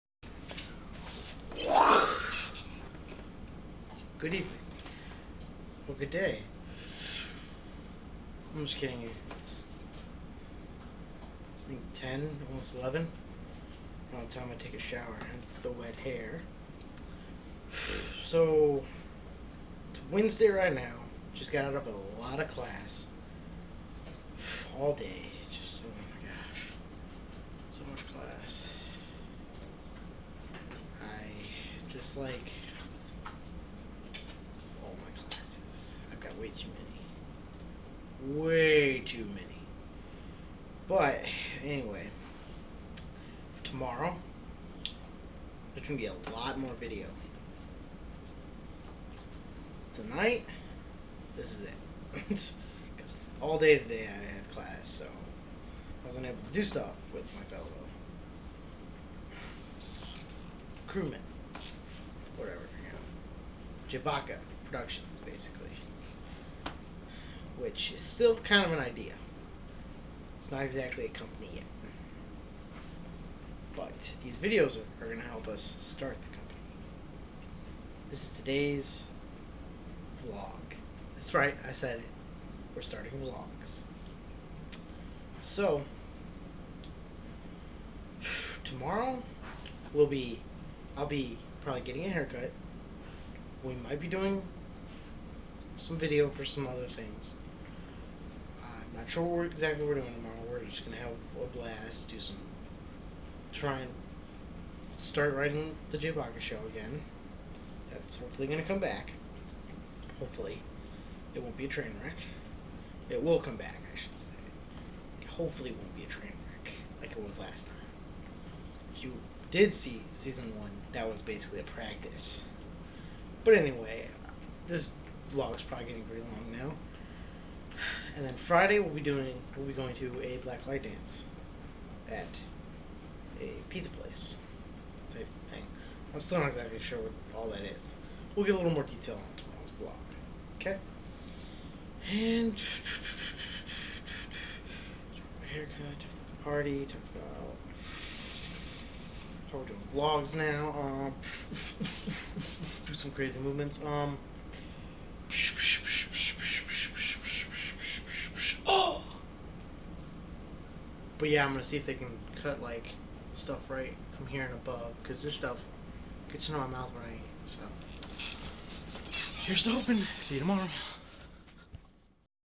JayBacca X Productions Prep Vlog 001
me talking into a camera about what im up to